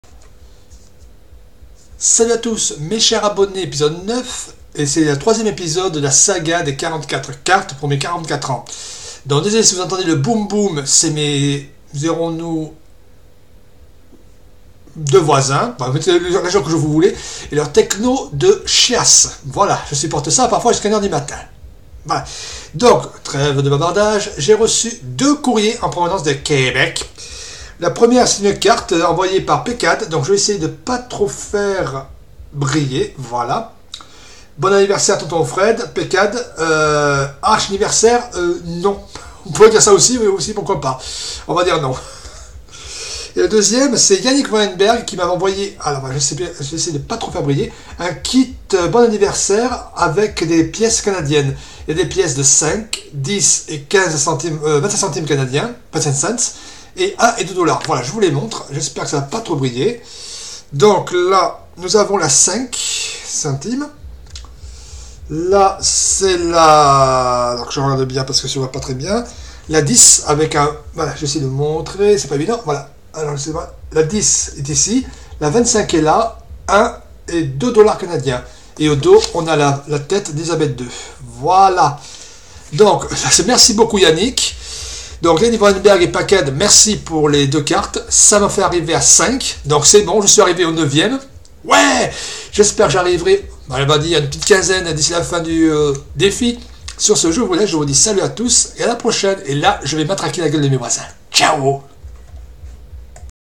Mes chers abonnés, épisode 9 : la saga des 44 cartes postales, épisode 3 :)
Un épisode qui sent bon le sirop d'érable, et oui, c'est un spécial Québec !

Merci à PkAdd et Yannick Volenberg !

Plus que 39 cartes au 2 février 2018 pour réussir le défi :)